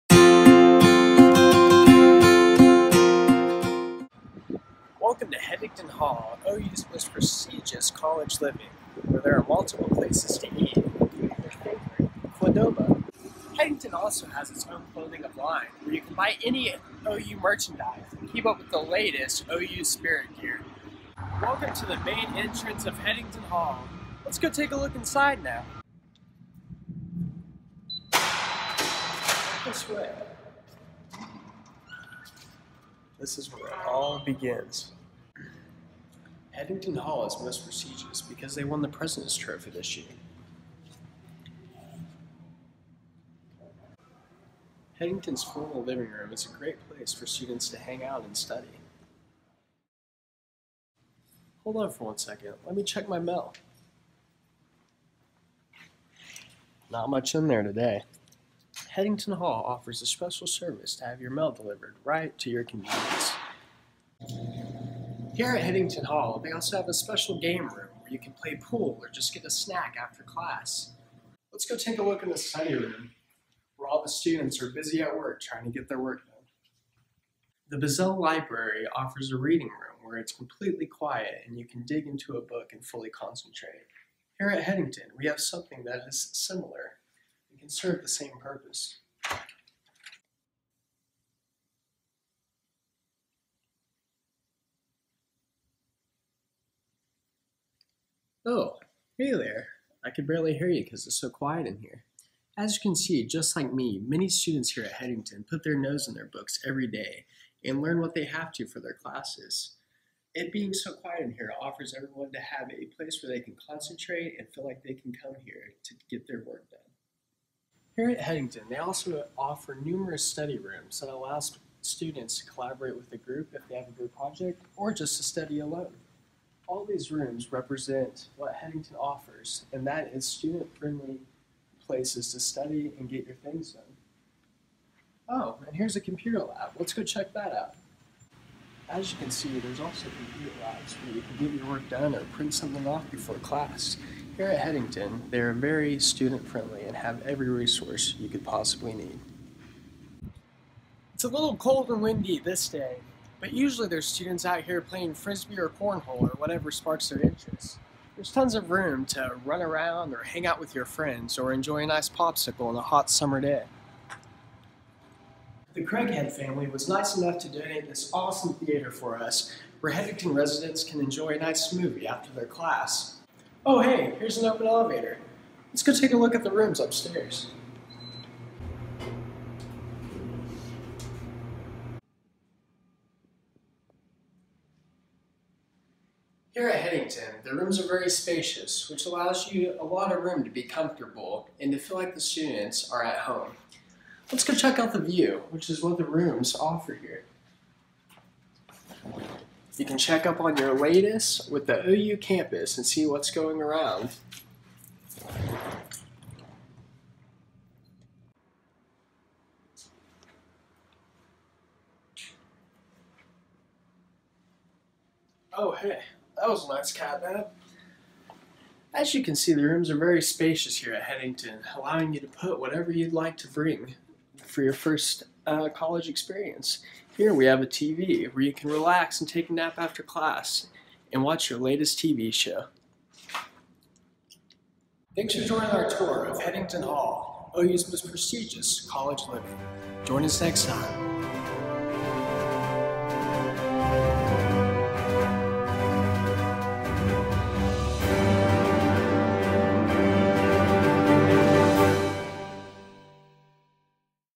Headington Hall Tour
This was a class project but I'm leaving it up because apparently people want a Headington Tour

Video Project #3 Professor Tarpenning